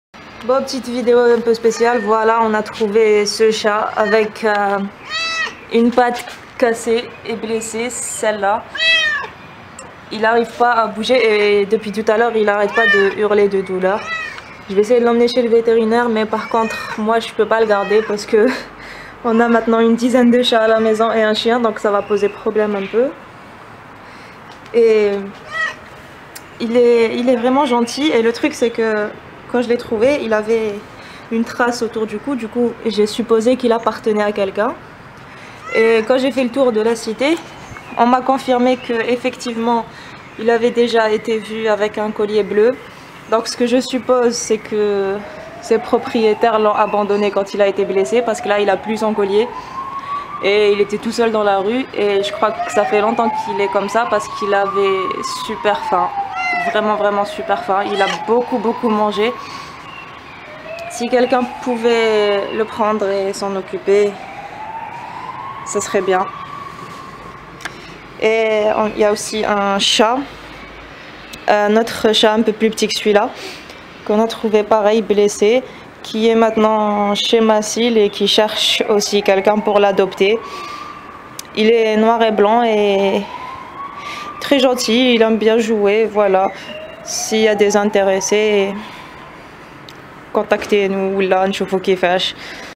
Adopte un chat
Contactez nous sur notre instagram si interessés merci, ces chats ont besoin d'une famille bienveillante et aimante.

Pouvait*

7chich kebab over chiche kebab